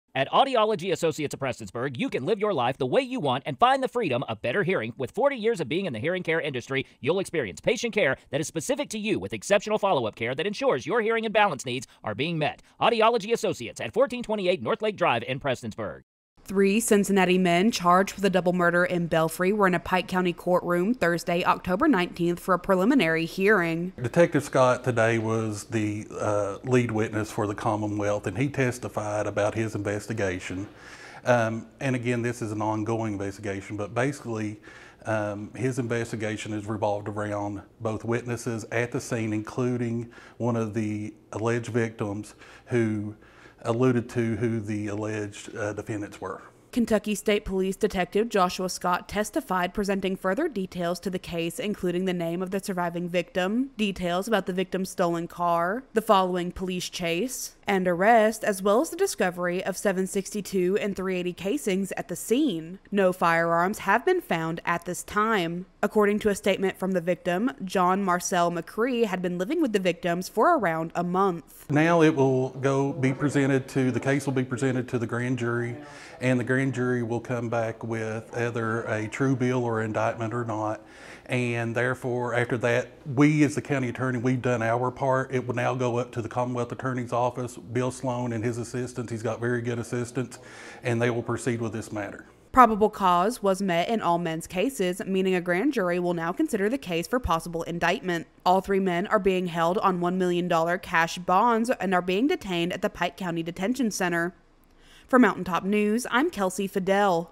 Preliminary Hearing Held for Belfry Double Murder
Three Cincinnati men who were charged with a double murder in Belfry were in Pikeville on Thursday, October 19, for a preliminary hearing.